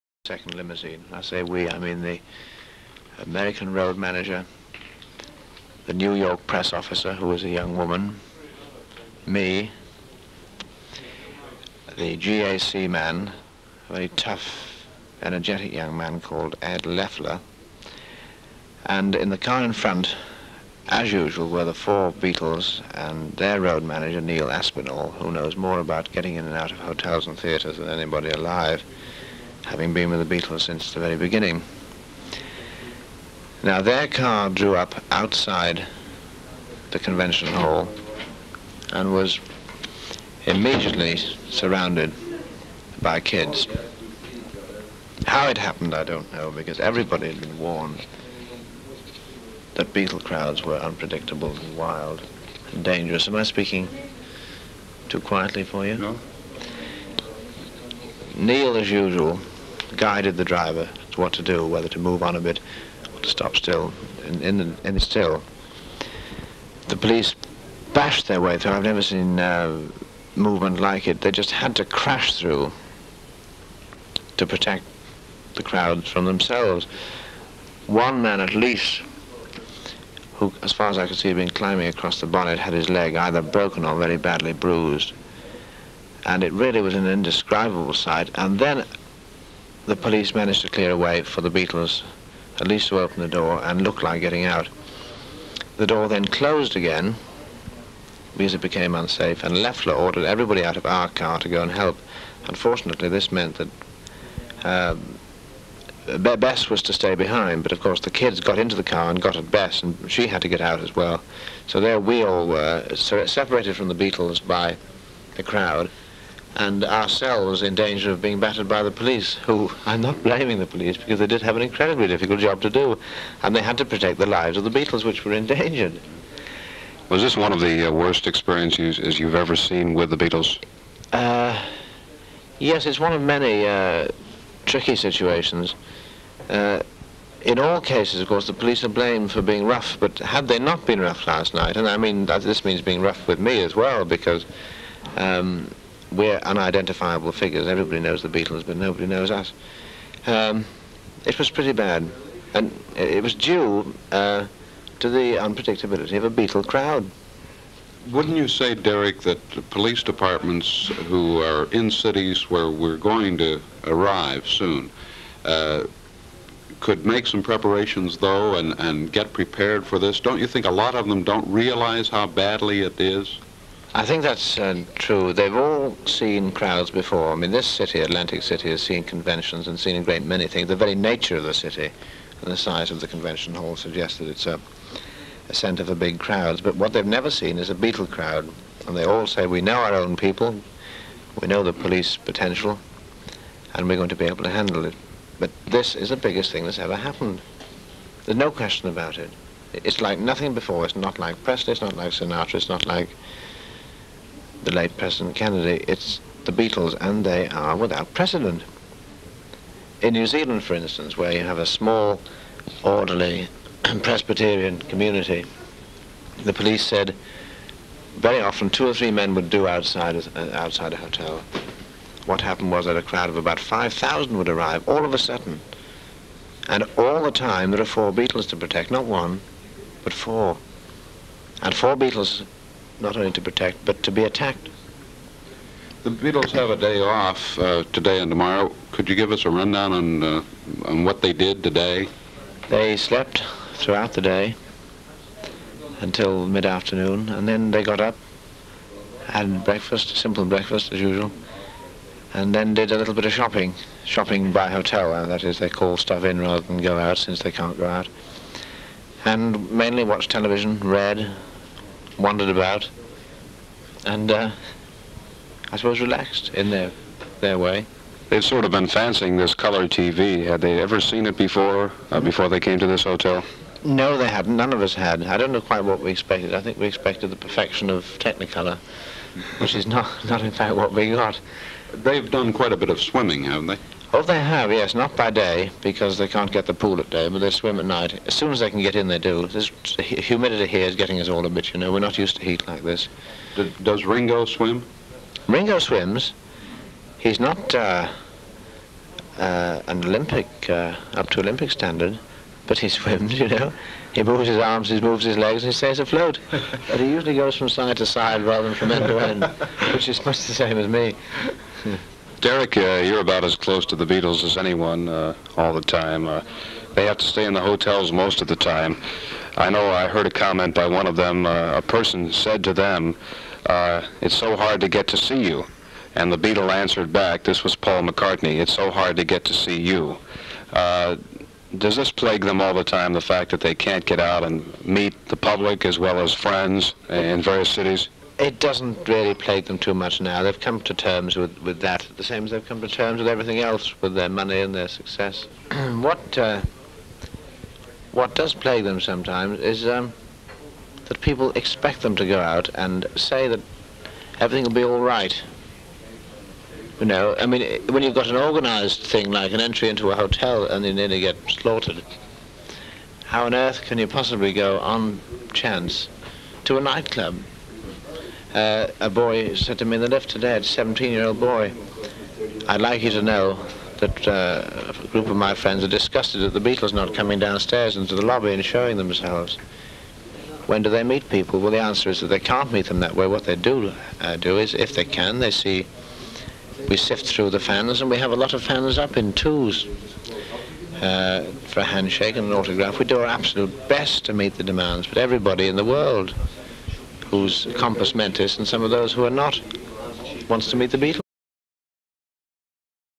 Press Officer Derek Taylor Discusses The Atlantic City Concert And The Police Protection On 1st...
Press Officer Derek Taylor Discusses The Atlantic City Concert And The Police Protection On 1st September 1964 · The Beatles

East Coast Invasion - Audiobook